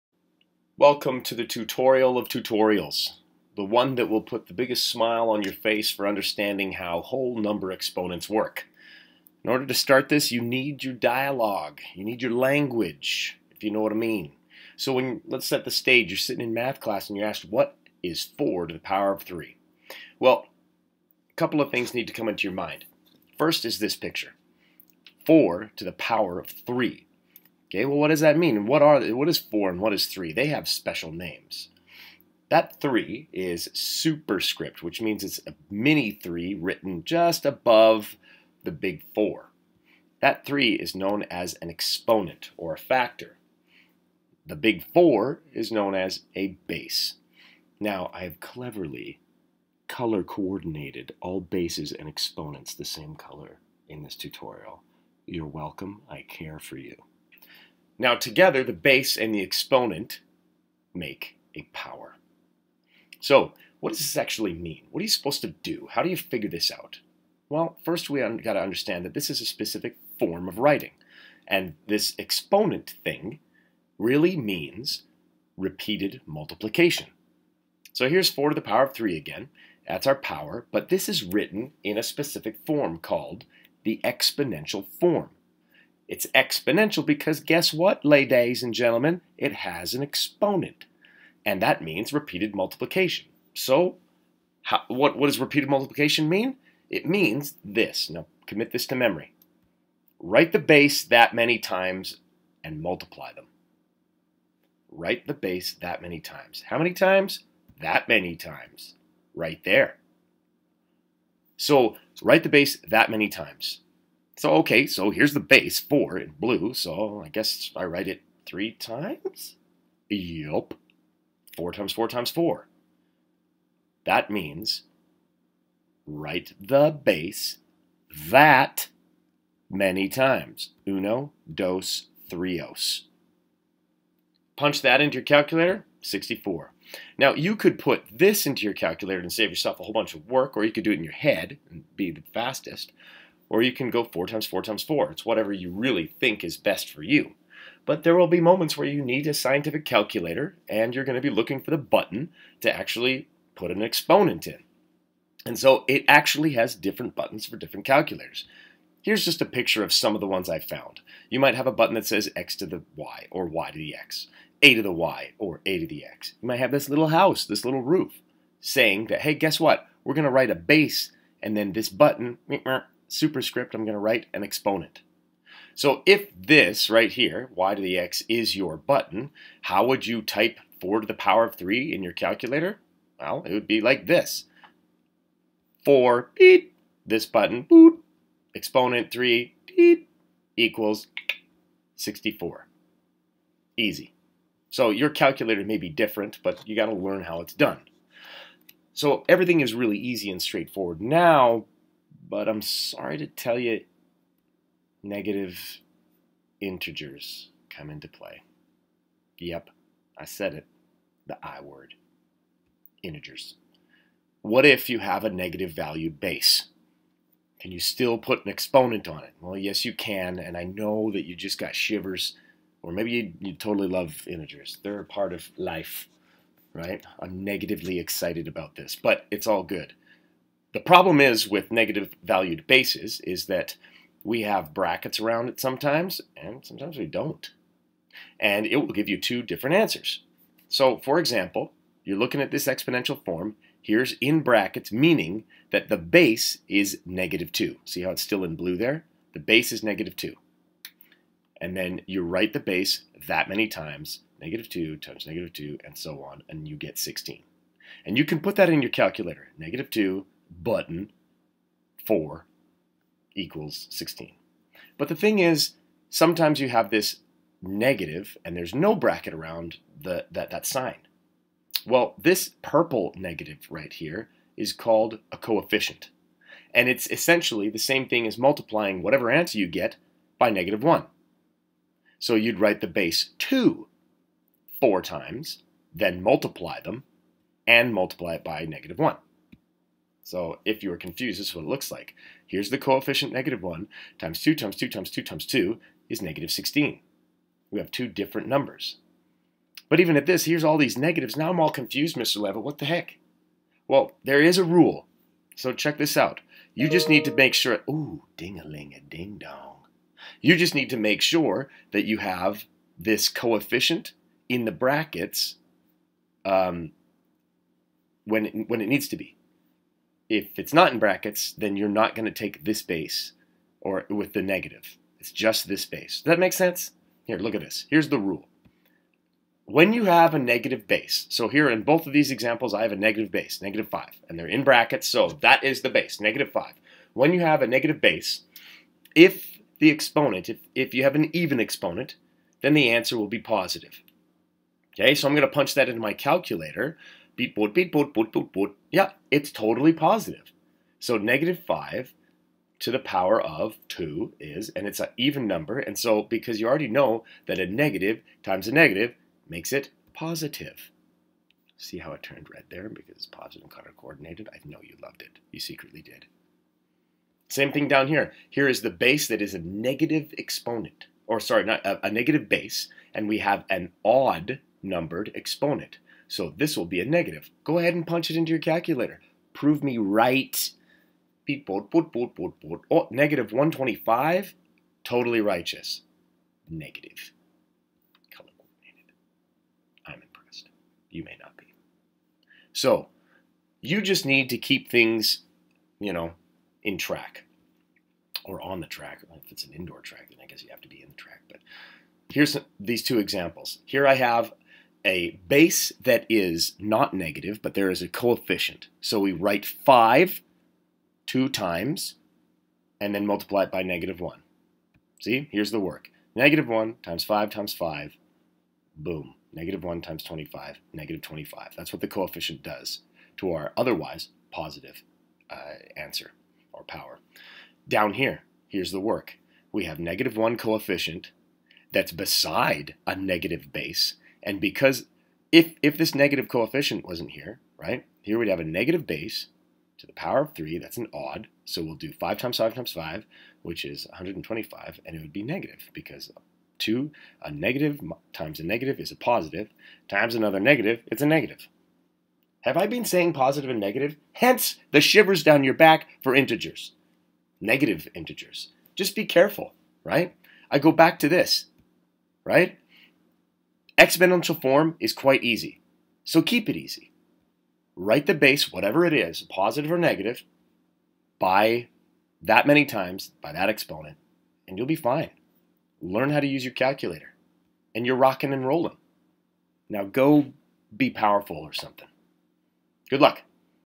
Exponent Basics (Whole s)
Here is the basics on exponents. What does it mean to ask, "What is 4 to the power of 3?" for example.  We go over the crucial language you need to know, and what to do when you have a negative base number.  This does NOT go through negative exponents.